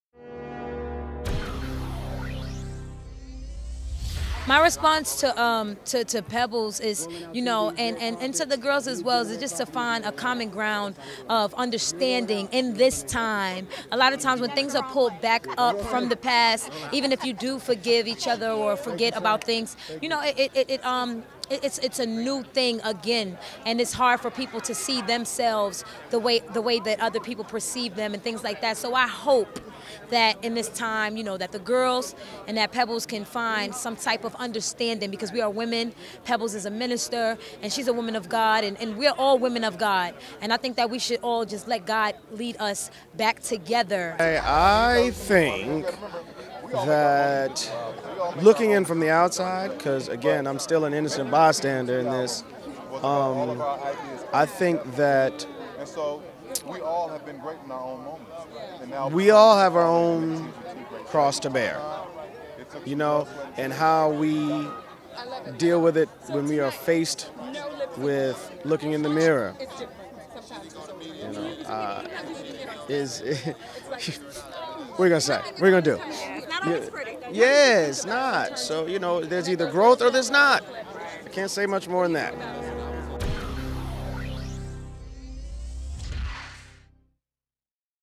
EXCLUSIVE: Lil Mama Responds to Pebbles and TLC Drama! - HipHollywood.com
Lil Mama, who played Lisa "Left Eye" Lopez in the biopic, isn't taking sides.  The raptress is asking for both sides to call a truce.  "I hope that in this time that the girls and Pebbles can find some type of understanding", Lil Mama told HipHollywood exclusively.  Even Carl Anthony Payne II who played L.A. Reid in the biopic gave his response to the drama.  Take a look at the full interviews.